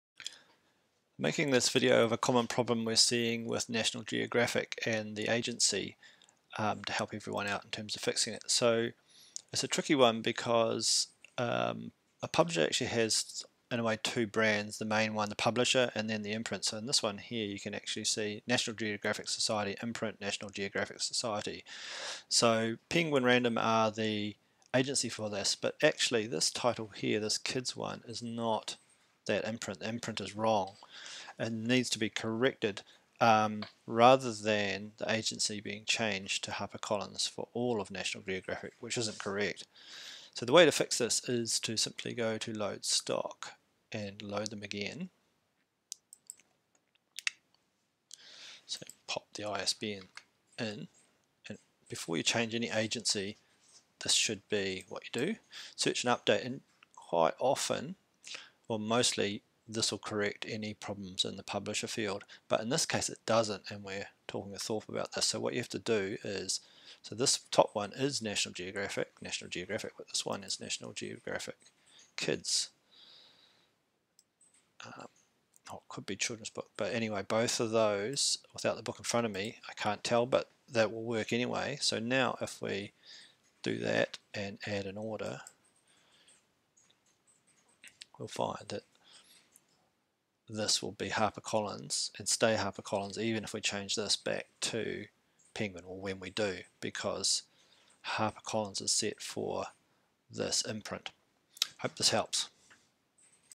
Fixing Imprint - National Geographic example
The source of most agency issues is incorrect imprint, this shows you how to easily correct it.